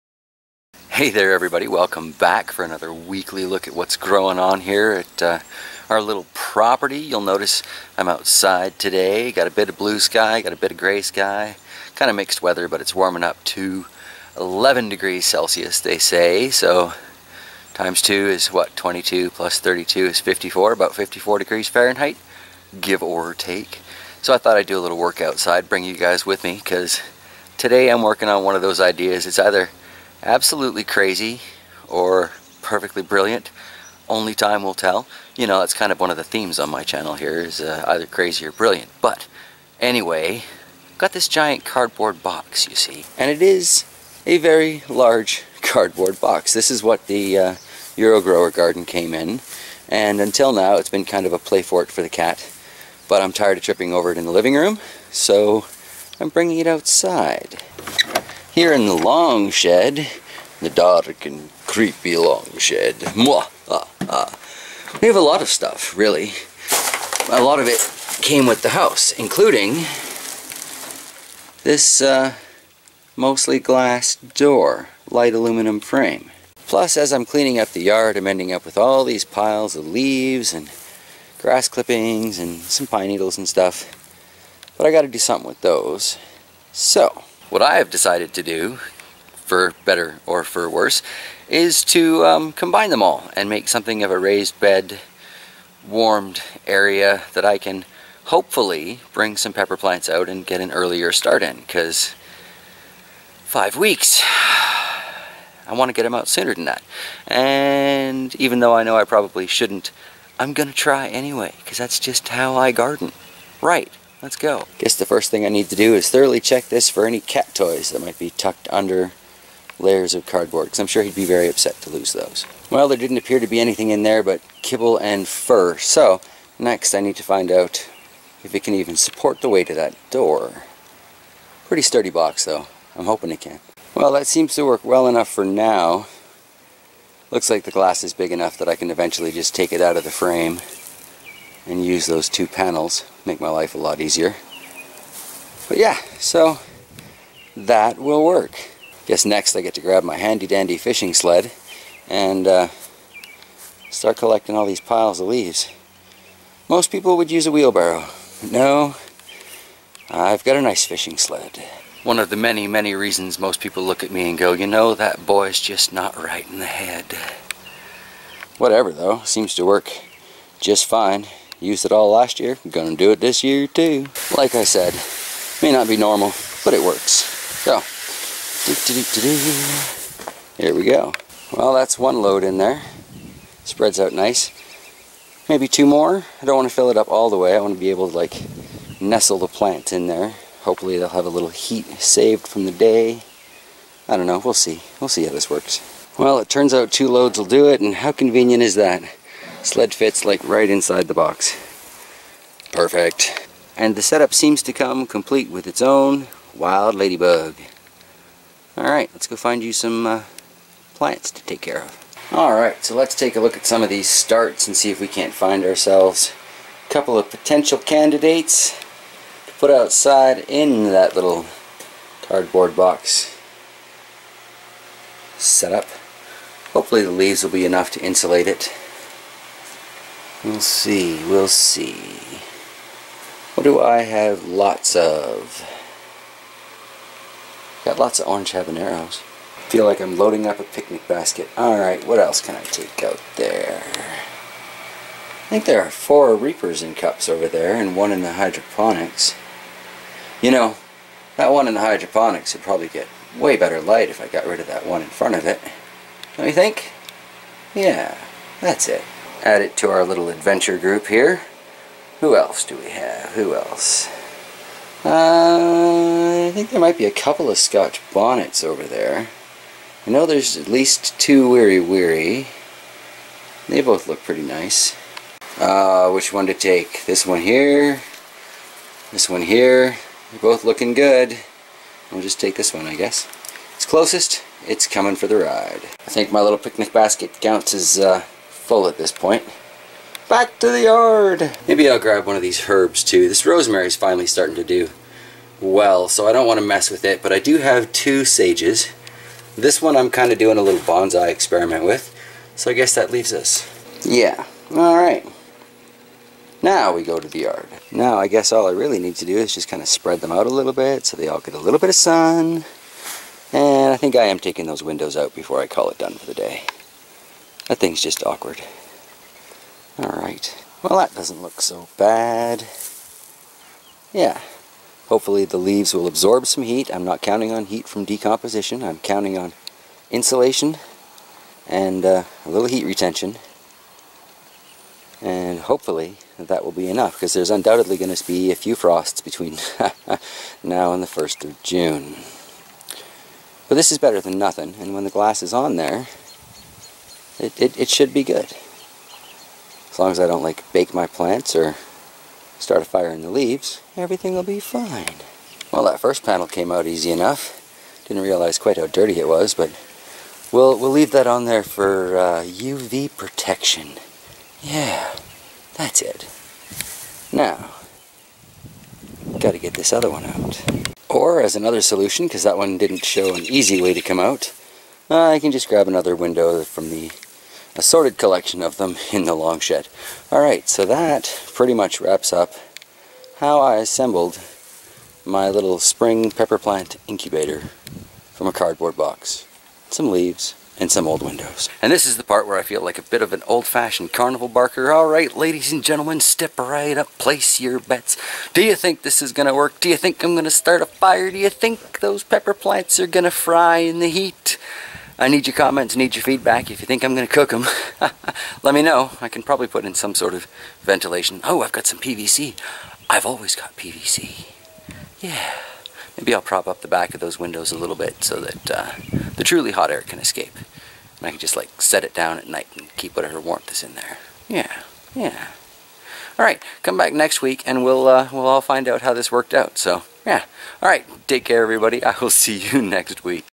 JT and the Cardboard Coldframe
Hey there everybody, I thought this week we'd do something a little bit different.  Since I don't yet have a proper coldframe to start putting my plants into I thought I would try making a quick, and temporary, one from this massive cardboard box that came with the hydroponic garden down stairs.  Waste not, want not they say ... well they said that to me growing up anyway ...
Ya, I have no idea if this will work out or not but if it does then I suspect I'll be doing this again in years to come!  Not like I'm going to be giving up gardening any time soon ...
Anyway, place your bets in the comment below on how you think this little cardboard coldframe will work and feel free to fill me in on any suggestions you may have!  I always love the feedback.  :-)
Have a great week ahead everybody!

Some of the other channels, if you're interested .